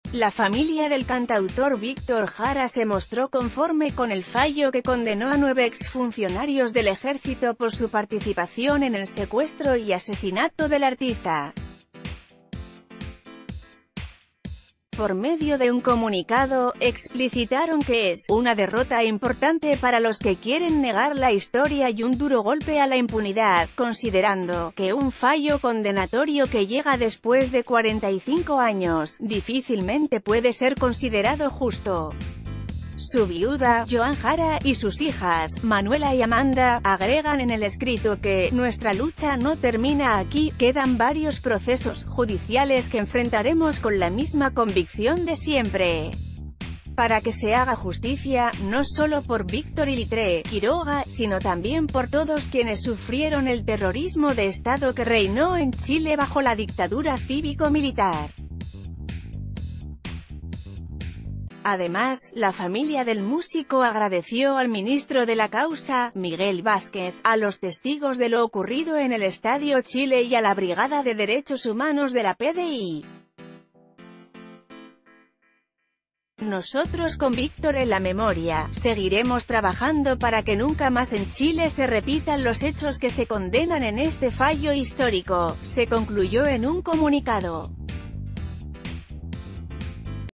Familia de Víctor Jara y condena a exfuncionarios del Ejército: "Nuestra lucha no termina aquí"
Cesar Guzman